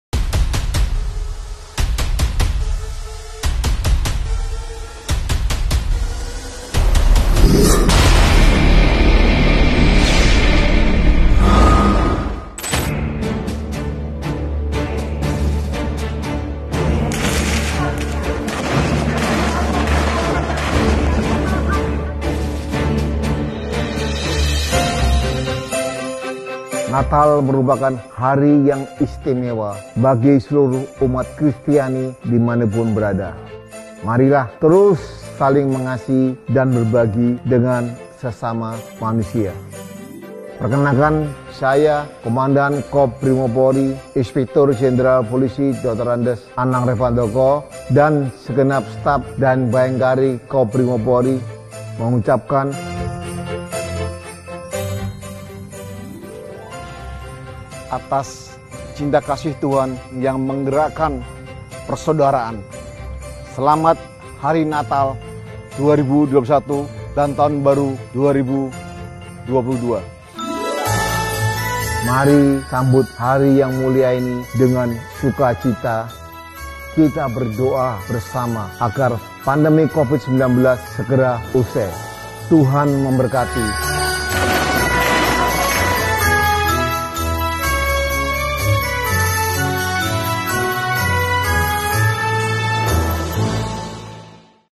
SELAMAT NATAL 2021 DAN TAHUN BARU 2022
Selamat Hari Natal 2021 dan Tahun Baru 2022.
Cinta Kasih Tuhan Menggerakkan Persaudaraan.

Perayaan Natal dan Tahun baru ini harus tetap patuhi protokol kesehatan agar Pandemi Covid-19 bisa segera usai.